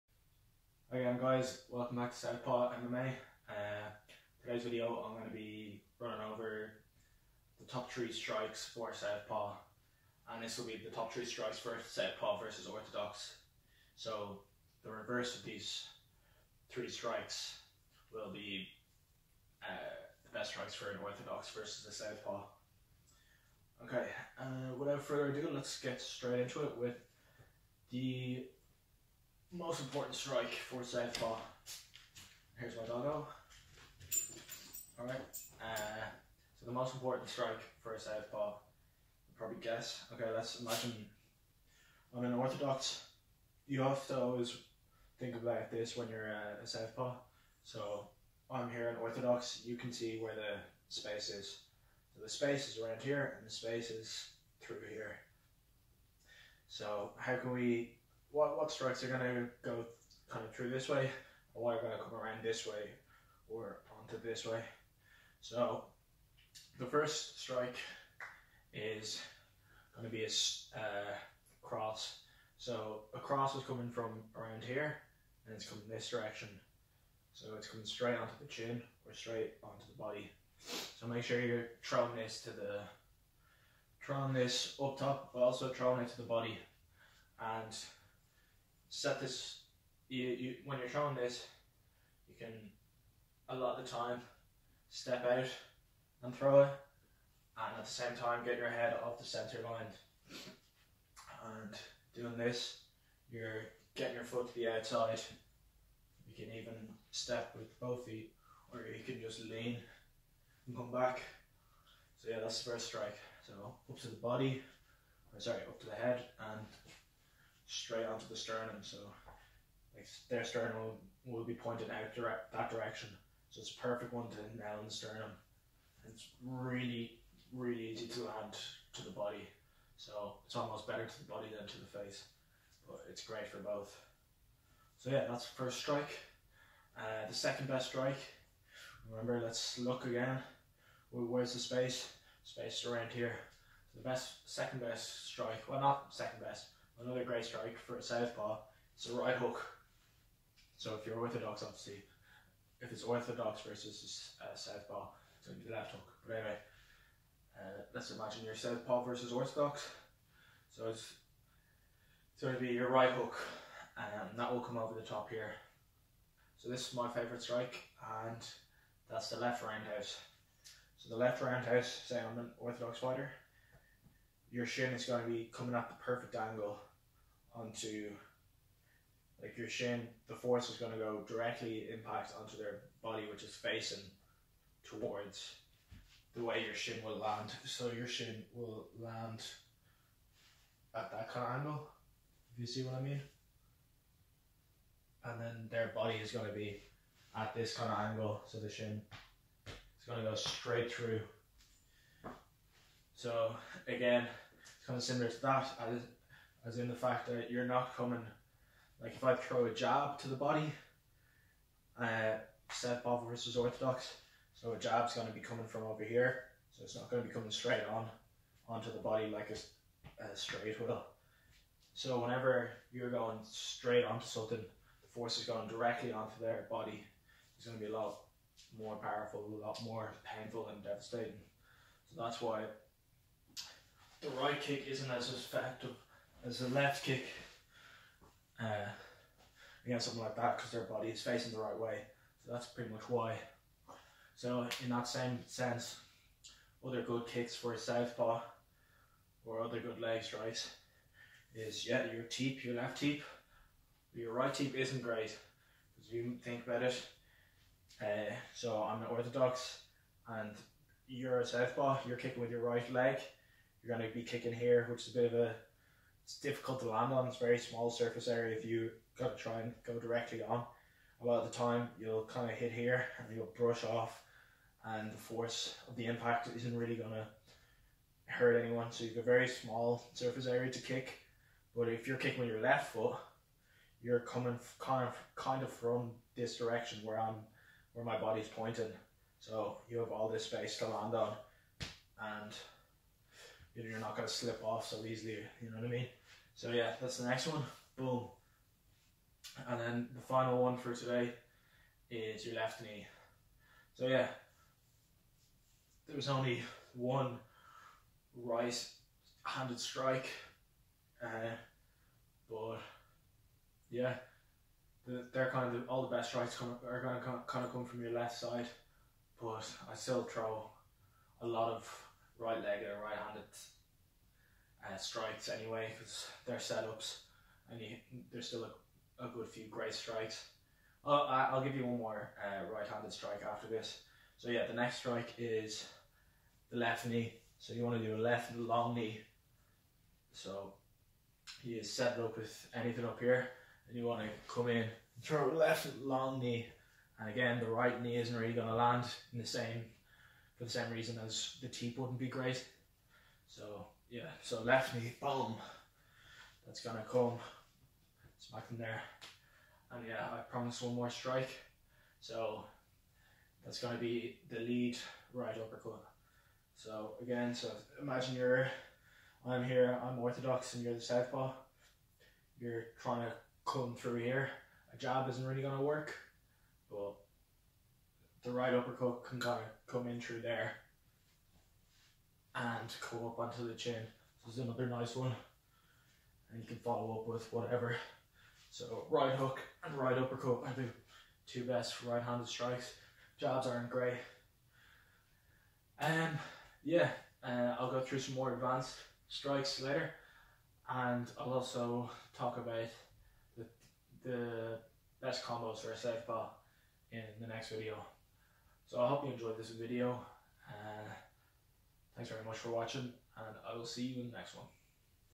The Best Single Southpaw Strikes
Running through the best strikes for a southpaw. If you enjoyed the video let us know, and if you didn't tell us what we could improve.

Follow Emile on his personal Instagram: @emileprior